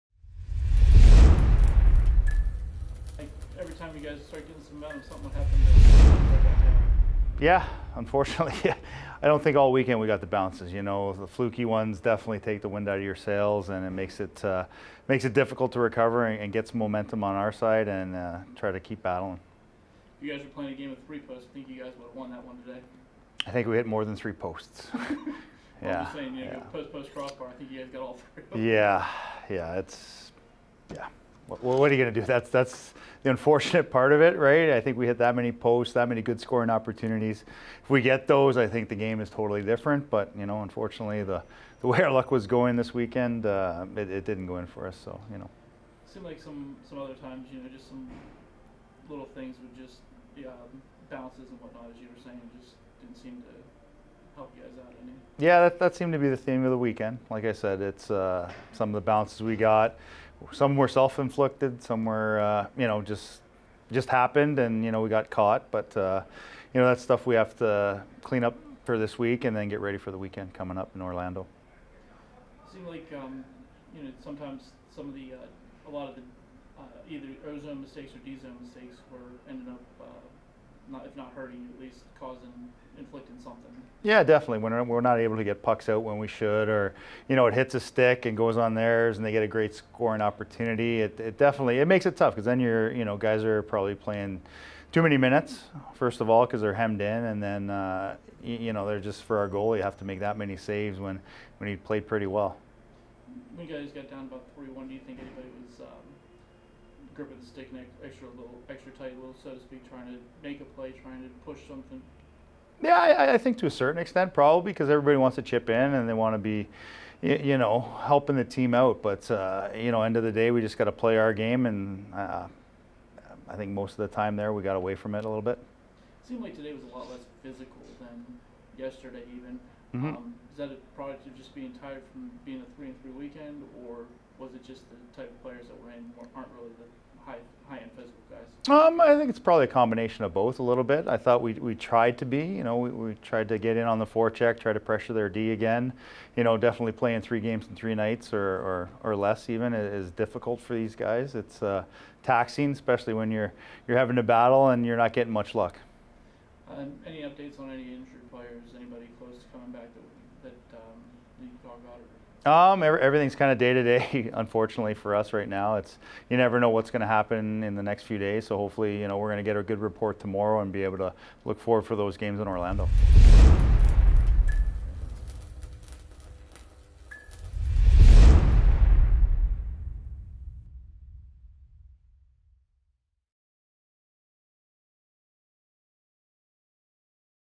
Ryan Kuwabara: Zambito Post-Game Press Conference 1-9-22
Ryan Kuwabara discusses tonight's game against the Kalamazoo Wings in this edition of the Zambito Post-Game Press Conference.